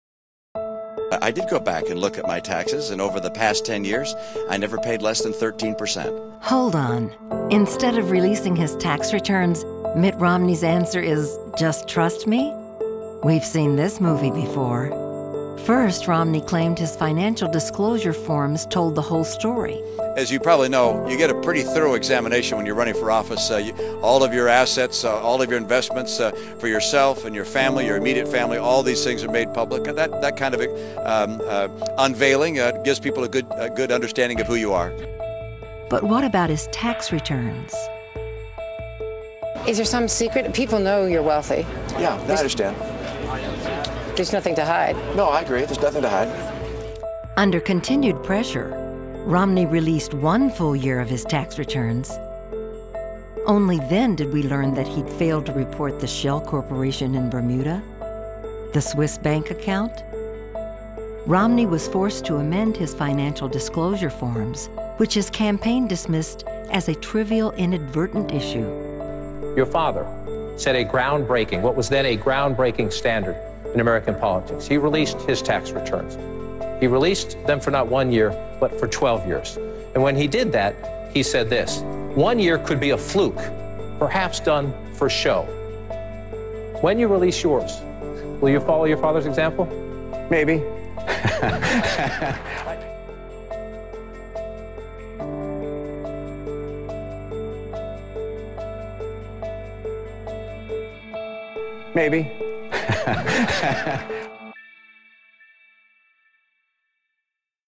Maybe
Over the course of the campaign, Romney assured the American public that his financial disclosure forms told the full story about his finances. Now we know they didn't -- when he released his 2010 tax returns, he was forced to amend those forms to include his offshore accounts in the Cayman Islands, Bermuda, and Switzerland.  Mitt Romney wants Americans to "just trust him" when he hasn't been forthcoming in his disclosure forms and his running mate, Paul Ryan, has proposed a plan that would reduce his tax burden to less than one percent, while raising taxes on middle class families by an average of more than $2,000?  Mitt Romney should come clean with the American public, explain what he's hiding and why he thinks it's fair that middle class Americans should pay far higher tax rates than he pays now and what he would pay under Paul Ryan's plan.